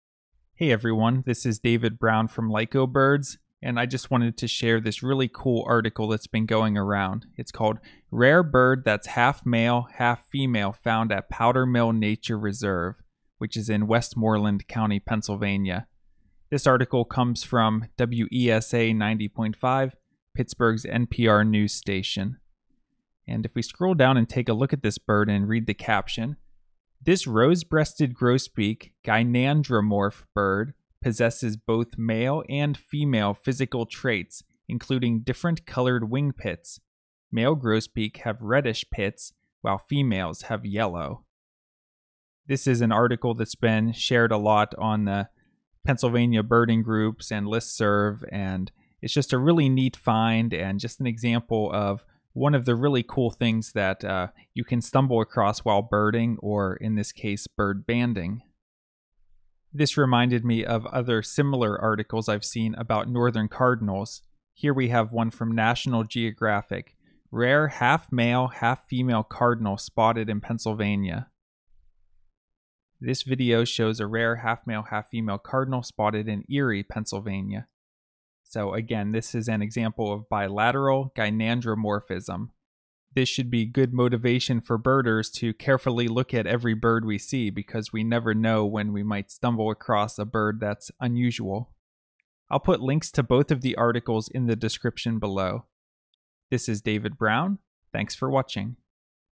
Half Male, Half Female Bird - Bilateral Gynandromorph Rose-breasted Grosbeak - Birding News
I share an article about a rare half male, half female Rose-breasted Grosbeak that was banded at Powdermill Nature Reserve in Westmoreland County, Pennsylvania, USA. I also discuss a second article that shows bilateral gynandromorphism in a Northern Cardinal.

Links:
Rare Bird That's Half Male, Half Female Found At Powdermill Nature Reserve